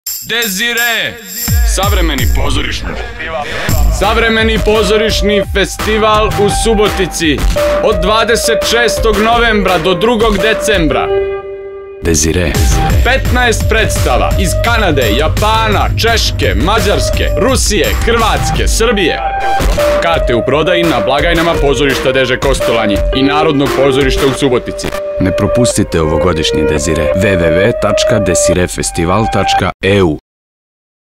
DESIRE 2011 - promo video (Serbian version) QDV.mov
Promo video for:
DESIRÉ Central Station 2011 - East,
contemporary theater festival organized by Kosztolányi Dezső theater from Subotica / Szabadka - Serbia

November 26th - December 2nd
Subotica / Szabadka - Serbia

Graphic design: Antal László
Video animations: Darko Maletin
Sound technician: Nebojša Adamov
Vocals: Srđan Sekulić (Serbian version) & Béres Márta (Hungarian version)
Texts: Dejan Vujinović / Szerda Zsófia
Concept & executive production: Mango marketing / Dejan Vujinović